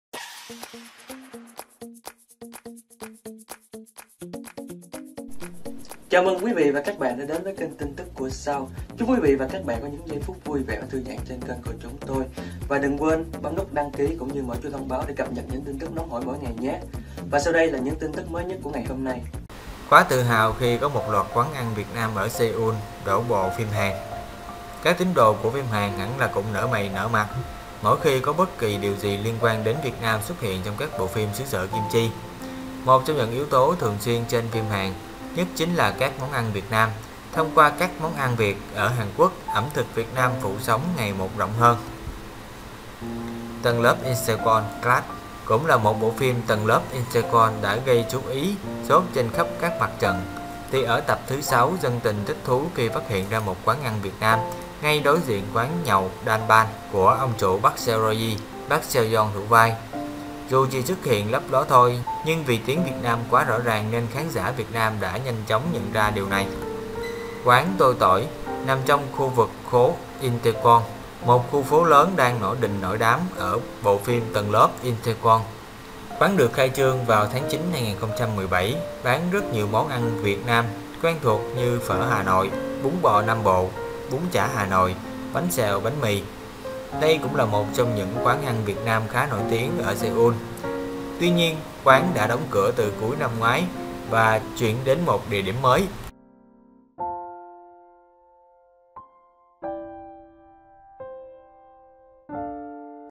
Qúa tự hào khi loạt quán ăn Việt đổ bộ phim Hàn
Qúa tự hào khi loạt quán ăn Việt đổ bộ phim Hàn. Tin tức của sao là kênh chuyên tổng hợp các sự kiện, tin tức của sao trong và ngoài nước.